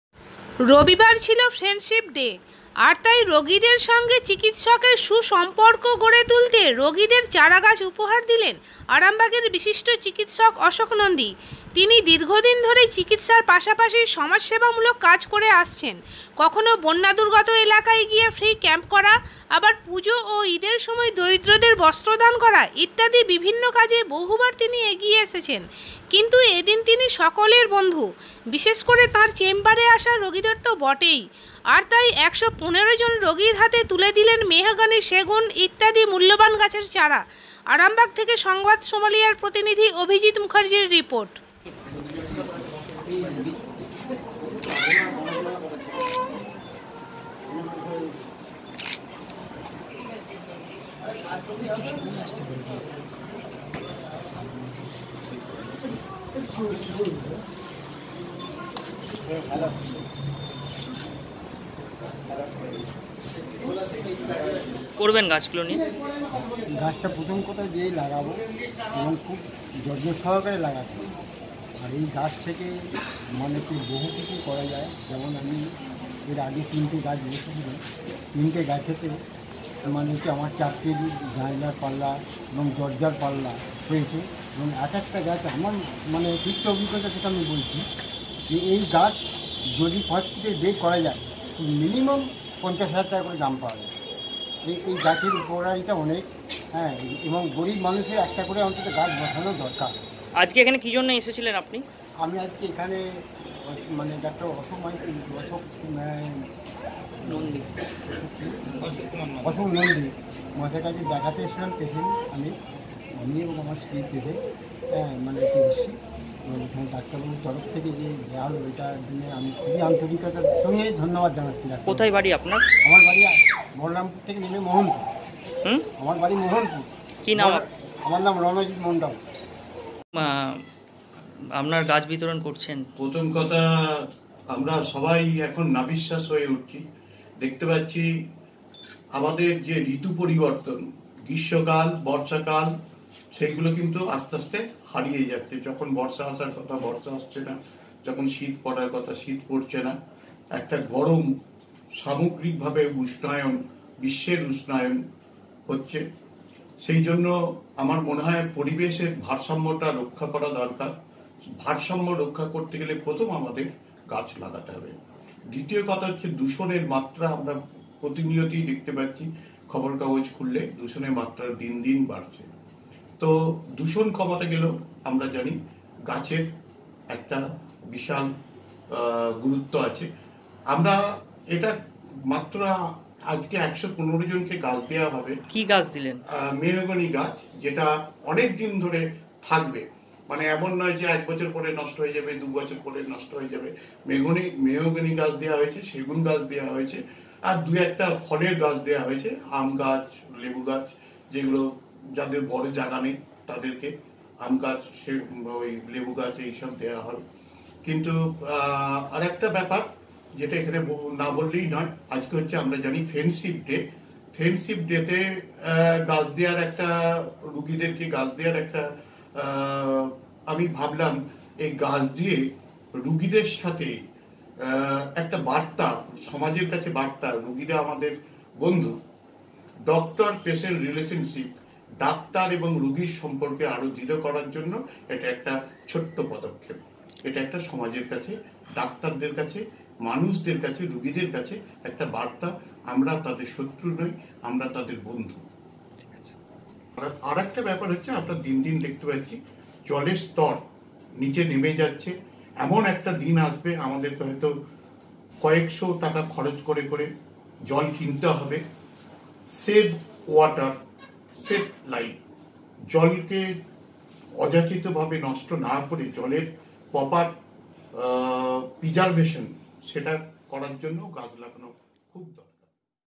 'ফ্রেন্ডশিপ ডে'-তে রোগীদের গাছের চারা উপহার দিলেন আরামবাগের বিশিষ্ট চিকিৎসক অশোক নন্দী
Sambad Somalia @ 'ফ্রেন্ডশিপ ডে'-তে রোগীদের গাছের চারা উপহার দিলেন
 আরামবাগের বিশিষ্ট চিকিৎসক অশোক নন্দী